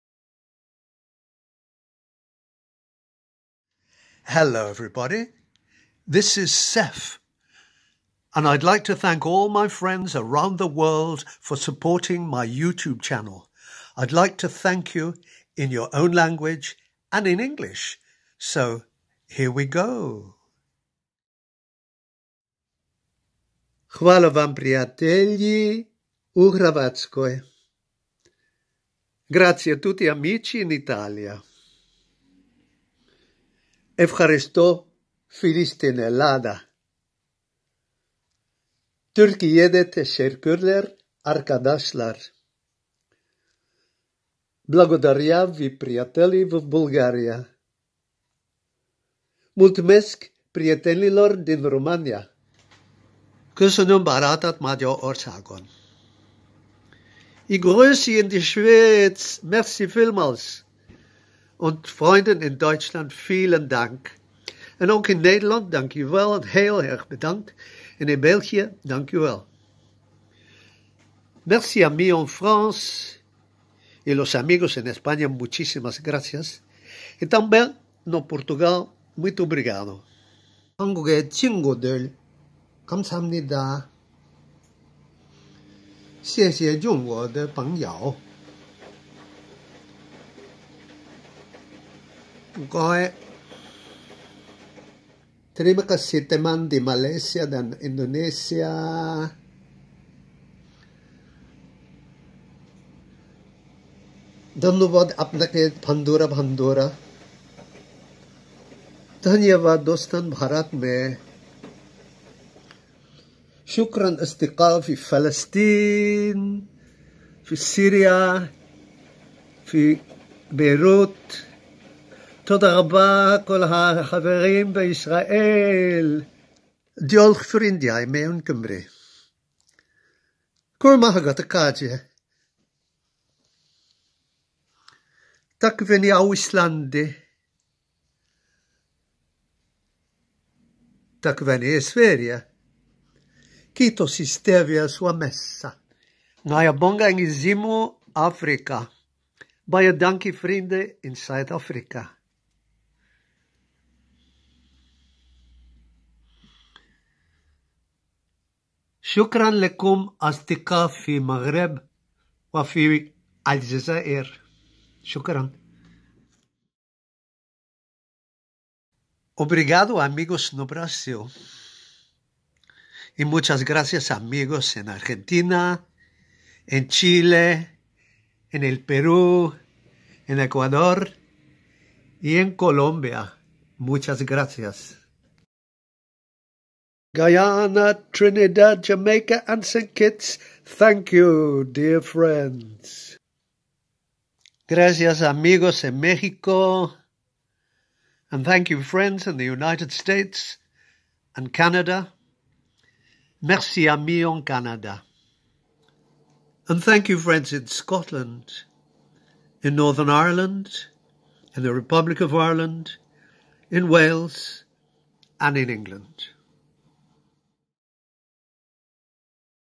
An International "Thank You"
An international 'THANK YOU' to a really international response.
Muchas Gracias- 謝謝- תודה- Hvala- Go raibh maith agat- شكرا جزيلا - Danke- Bedankt- Obrigado- Grazie- Ευχαριστώ- Merci- Nais tuke- Kiitos- 감사합니다- Diolch- Tack- ধন্যবাদ- Mulțumesc- આભાર- Köszönöm- Takk fyrir- Teşekkür ederim- благодаря